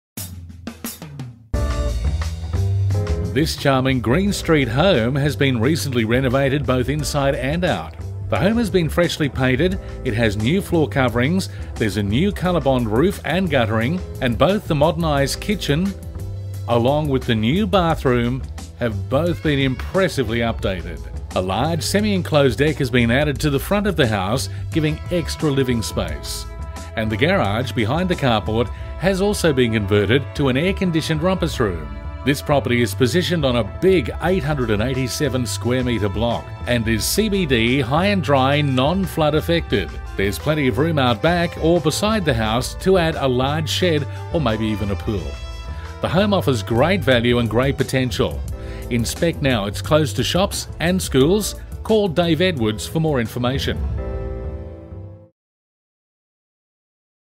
REAL ESTATE MACKAY - SOLD: 11 Green Street NORTH MACKAY - David Edwards - Ray White Mackay City
SOLD - by David Edwards for $320,000.00
Advert - This charming home has been recently renovated inside & out, freshly painted, new floor coverings, new colourbond roof & guttering, both the Modernised kitchen along with the New bathroom have both been impressively updated. A large semi-enclosed deck has been added to the front of house for extra living space & the garage, behind the carport, has been converted to an air- cond rumpus room Positioned on a big 887sqm block & CBD
Highndry-Non flood effected, theres plenty of room out back or beside the house to add a large shed and/or pool. This home offers great value, inspect now!

Close to schools, shops; Call David Edwards NOW on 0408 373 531